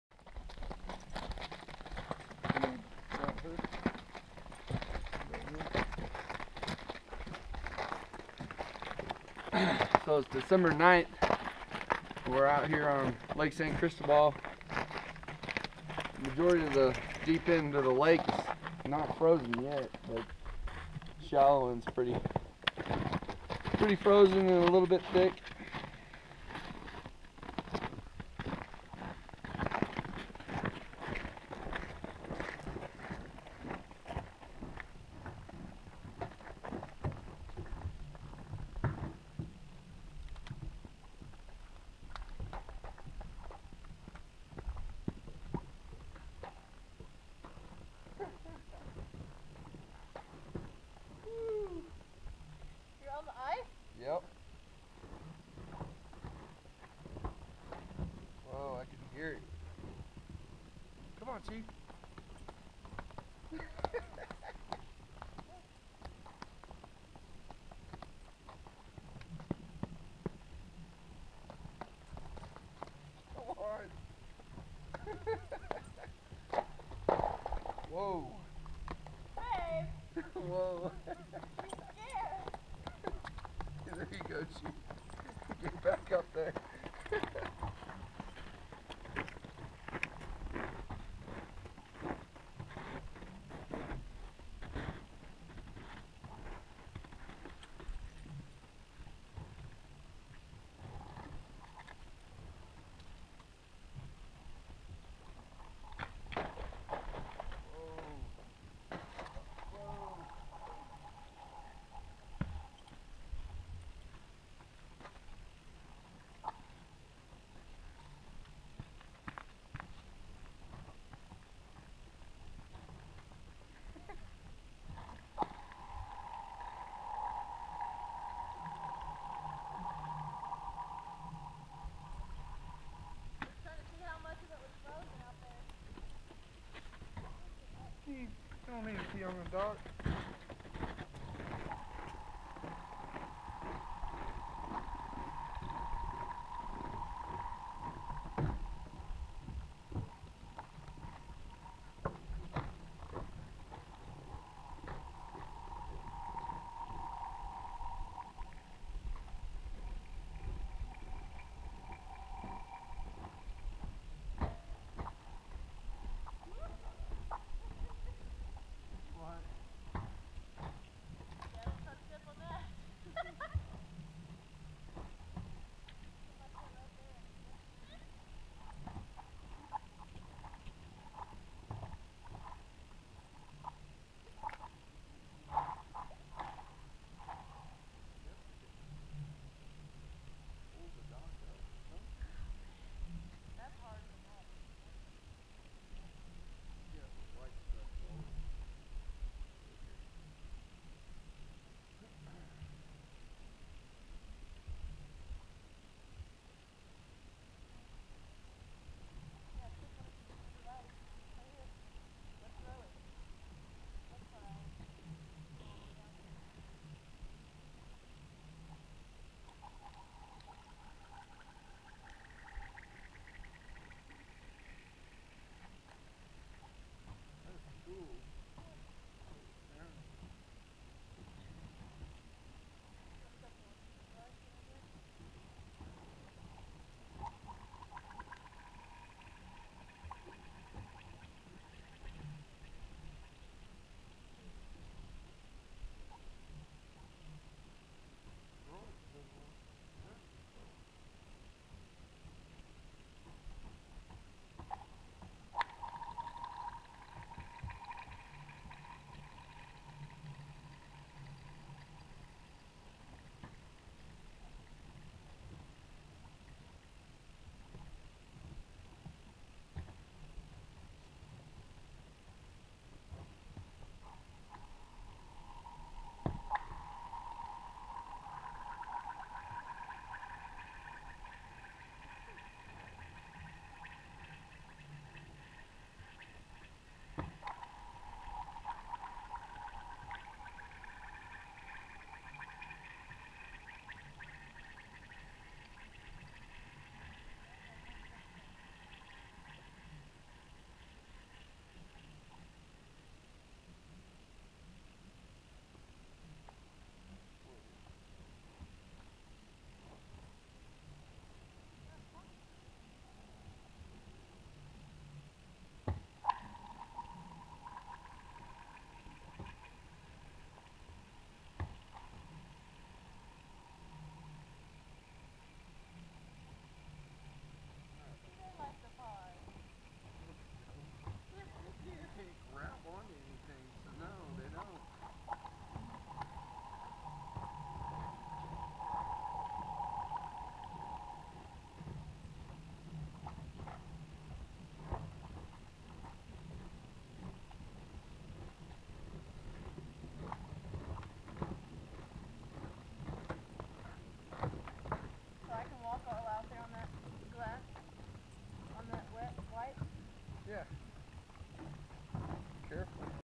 Lake San Cristobal Co.
Honeymoon to Lake City Colorado, Always love to spend time on this lake.. Use to live in a Small Cabin on this lake when I was 3 years old.